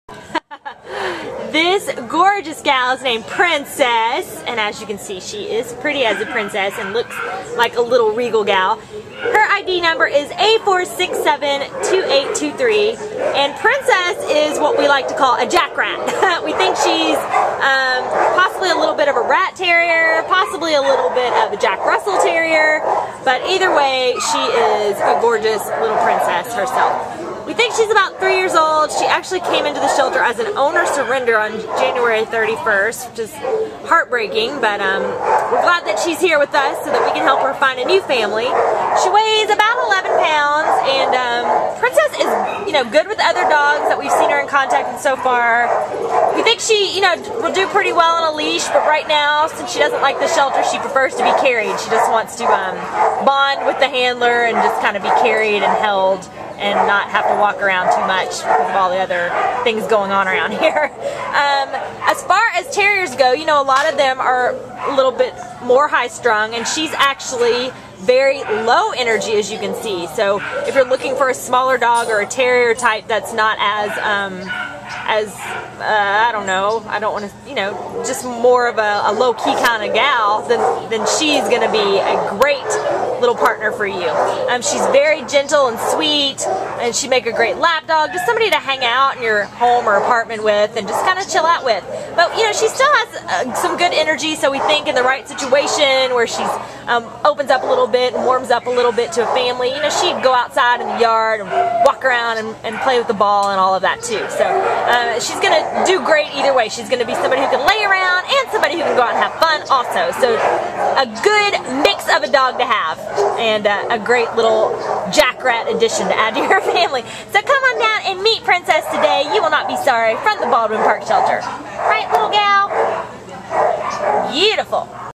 Princess A4672823
A4672832 New! Princess is an easygoing three-year-old female tricolor Jack Russell and Rat Terrier mix who was surrendered to the Baldwin Park Animal Care Center on January 31st because her owners could not afford the cost of caring for a dog. Weighing 11 pounds, Princess is a sweet and gentle even-tempered lady who is timid around people in the shelter environment at first but warms up quickly when shown a little attention and affection. She is a quintessential lap dog, walking decently on leash but preferring to be held and carried about like her name suggests! This adorable doll will make a darling indoor pet for any individual or family with school-aged children (who will be careful not to injure her small frame) in any living situation.